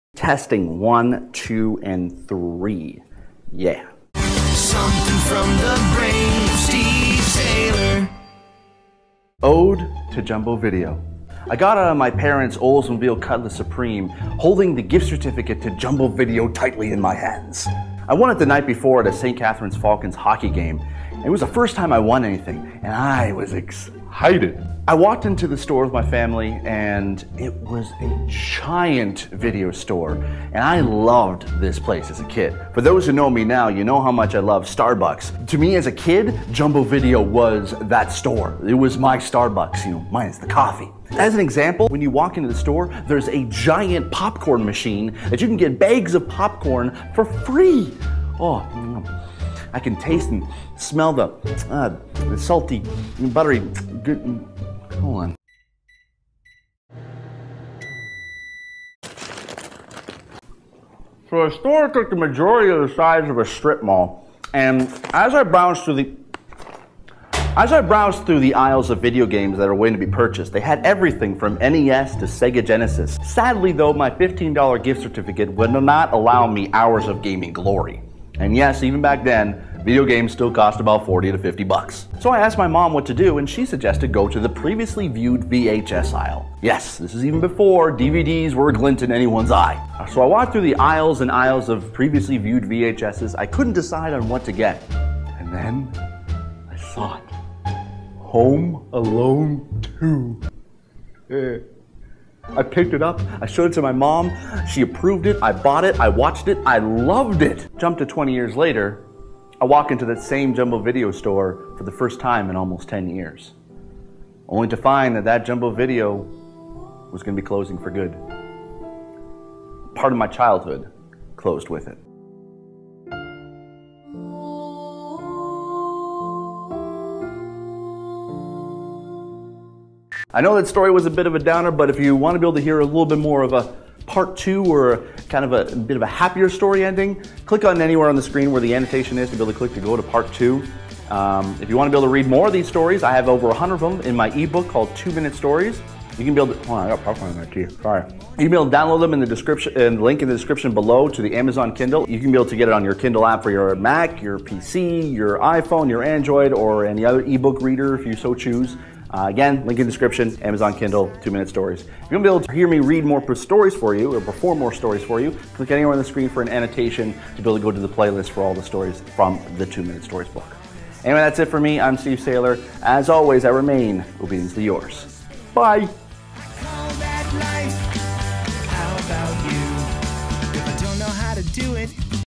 Ode To Jumbo Video
A Two Minute Story by Steve Saylor
BUY 2 MINUTE STORIES ON AMAZON!

Do you have a video store that you loved as a kid that has closed down?

As always I remain, obediently yours!